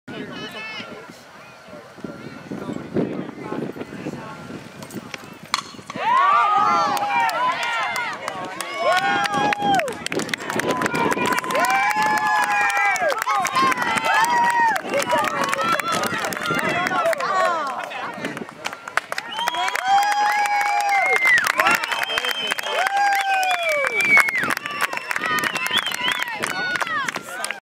Rachel Yax #55
Rachel Yax is a Junior from Iroquois High School and UA Avalanche 18u. Rachel is a power hitting Catcher / 1st Base.